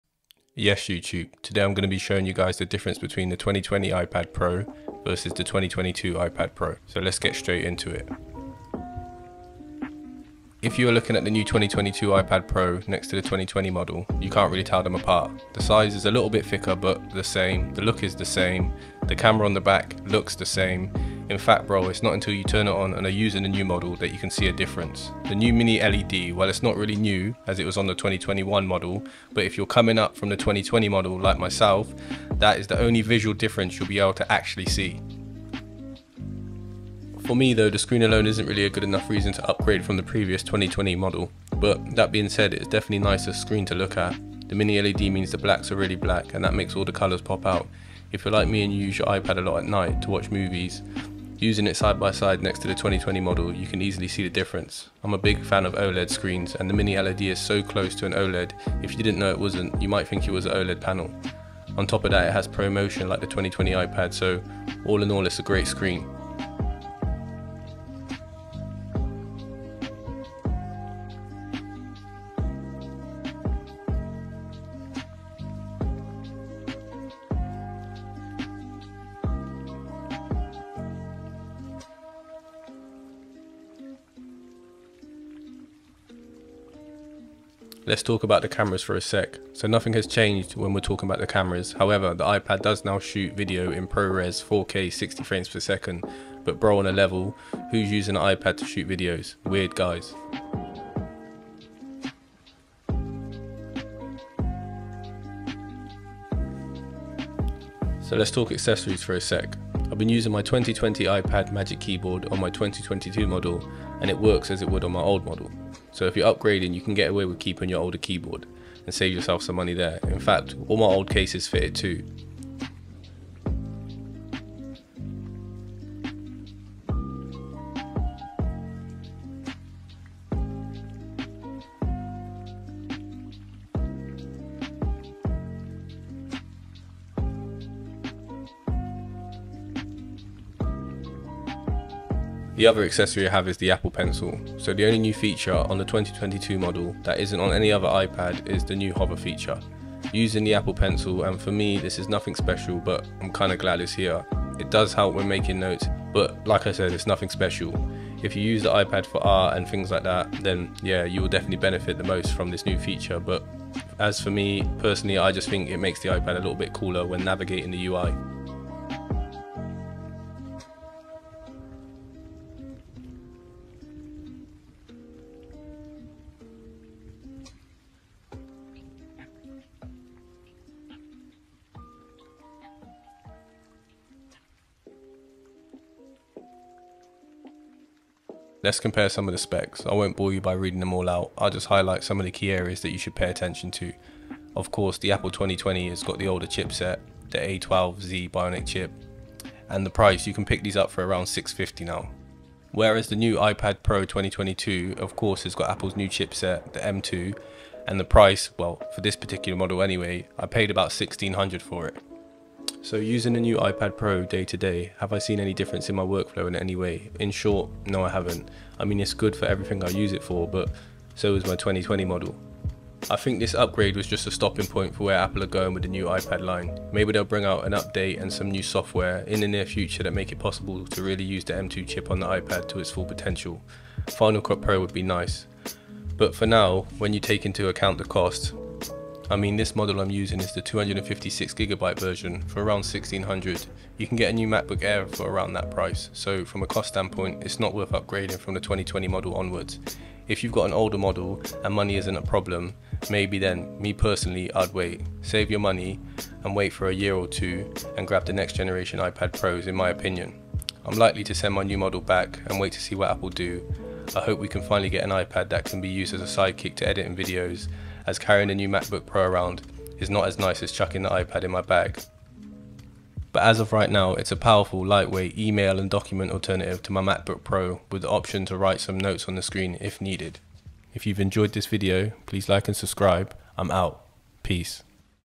iPad Pro 2022 (M2) vs iPad Pro 2020
Yes, youtube this is my first product review the iPad pro-2022 harder than it looks man but I hope you lot enjoy the video any feedback is appreciated just drop me a comment and subscribe. K

(Side note) Love for all the support so far man humbling foreal. 👊🏾